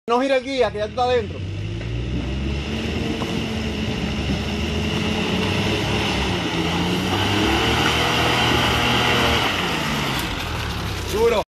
Mitsubishi Montero Pajero 2002 Off Road
- Mitsubishi Montero Pajero 2002.
- 3.5 V6.
- Off road in 4H.
Dominican Republic.
- Stock suspension (OME Lift coming soon) .
- Tires: Firestone Destination A/T.